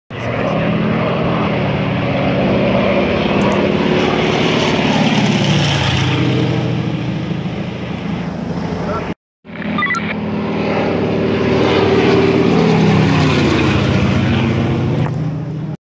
Zrzut wody przez dwa dromadery - ćwiczenia pk. "Las Kup"- KW PSP Opole
Komenda Wojewódzka PSP w Opolu